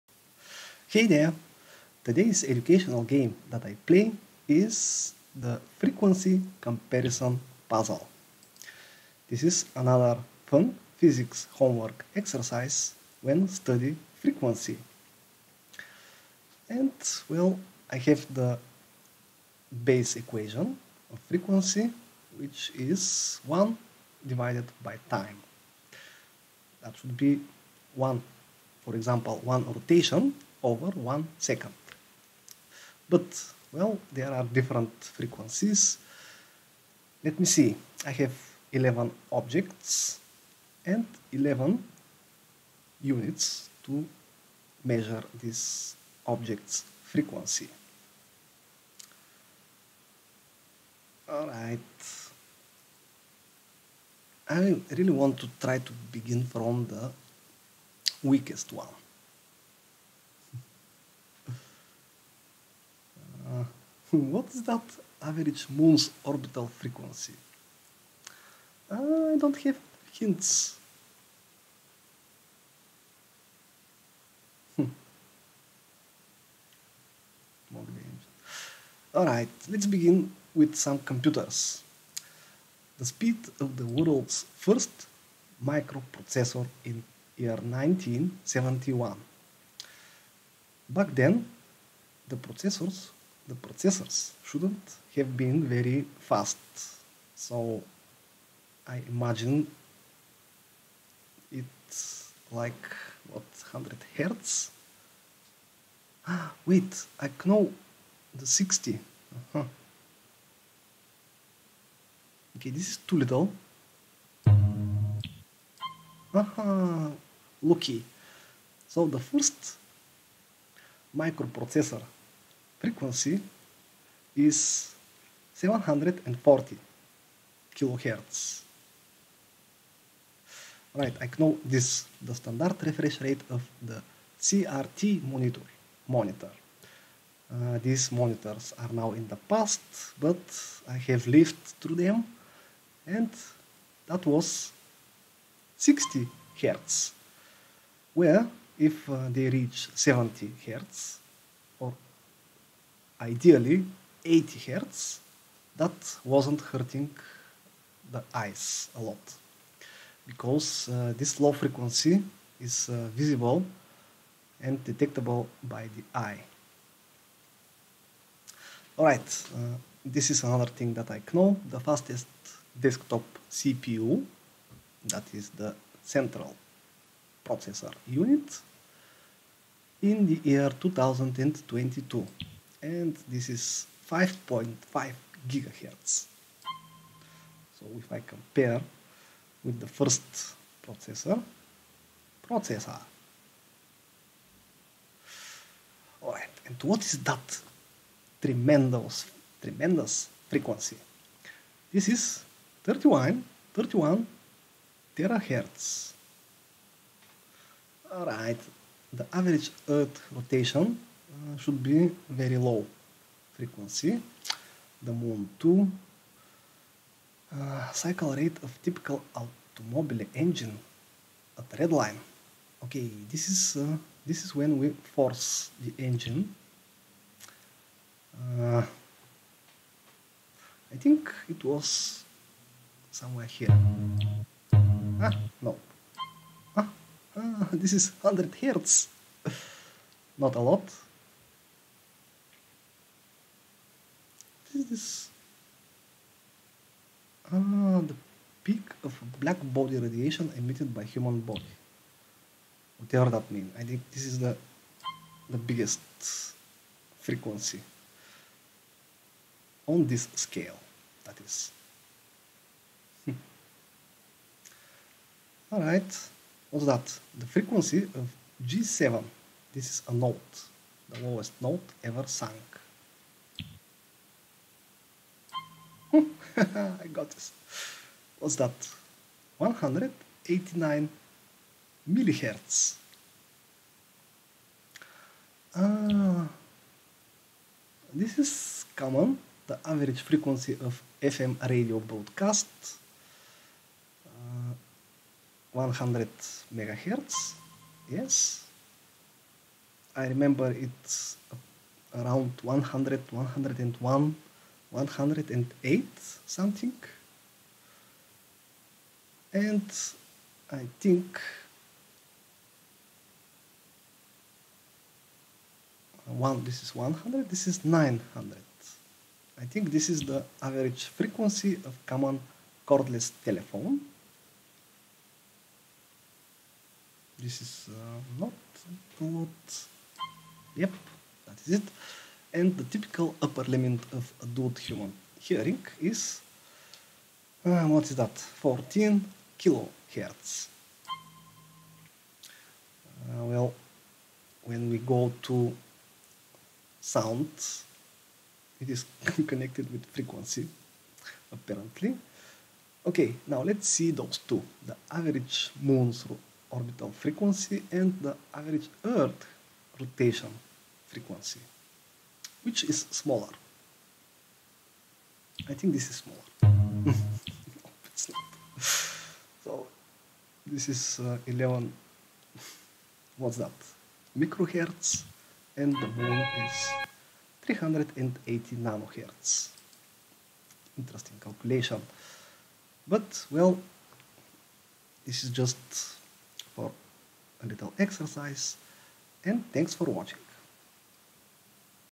Frequency Comparison - Interactive Physics Lesson 🚀🎥🎓
Fun educational video about the objects by their correct temporal frequency in order of magnitude. Suitable for online lessons, interactive classes and exciting homework.